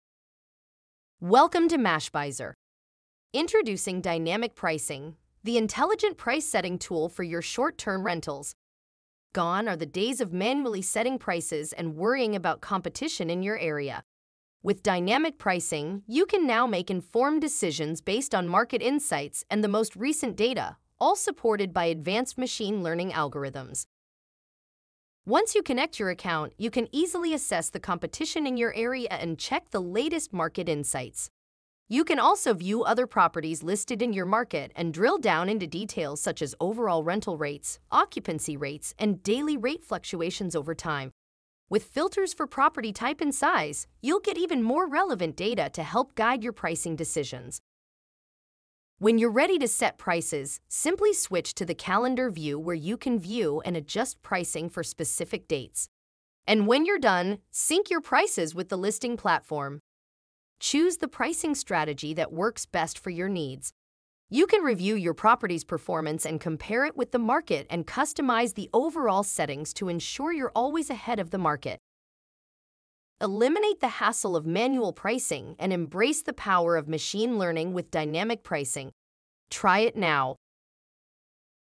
Dynamic Pricing - set your short-term rental pricing automatically
Introducing Dynamic Pricing - the intelligent price-setting tool for your short-term rentals.

Gone are the days of manually setting prices and worrying about competition in your area. With Dynamic Pricing, you can now make informed decisions based on market insights and the most recent data, all supported by advanced machine learning algorithms.